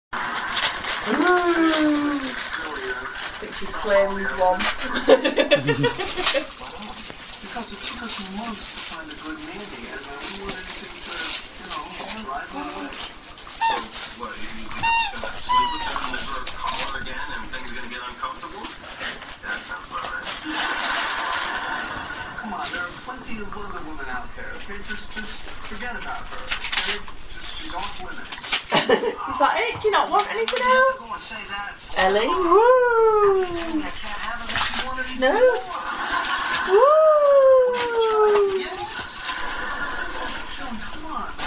Doggy christmas part 2
Elly and Snoops opening pressies